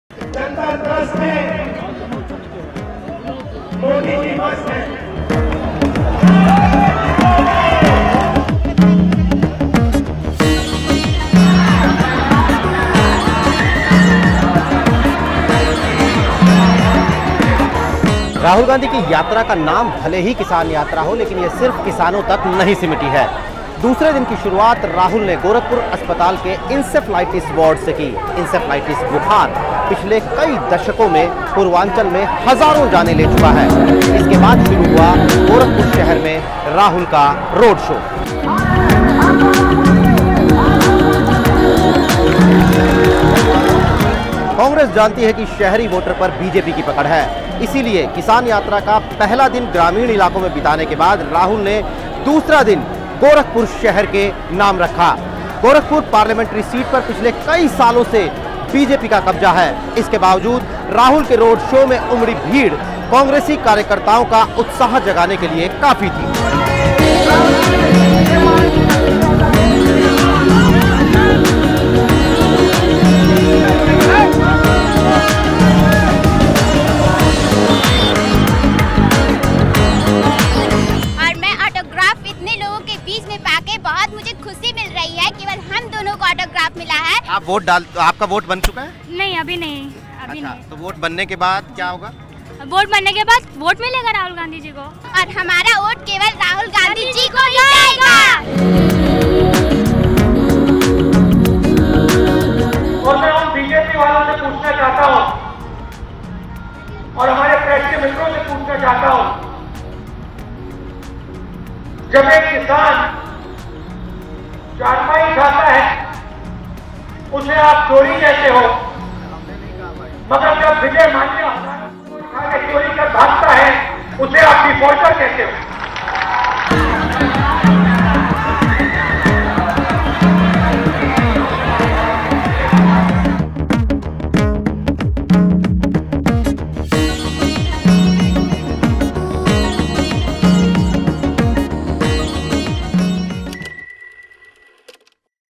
Quint Hindi: राहुल की किसान यात्रा, दूसरा दिन
यूपी में अपनी महायात्रा के दूसरे दिन राहुल गांधी बीजेपी और मीडिया के लोगों पर बरसे. उन्होंने कहा कि जब एक आदमी खाट ले जाता है तो आप उसे चोर कहते हैं. लेकिन अगर एक आदमी 9 हजार करोड़ लेकर भाग जाता है तो वो डिफॉल्टर कहलाता है.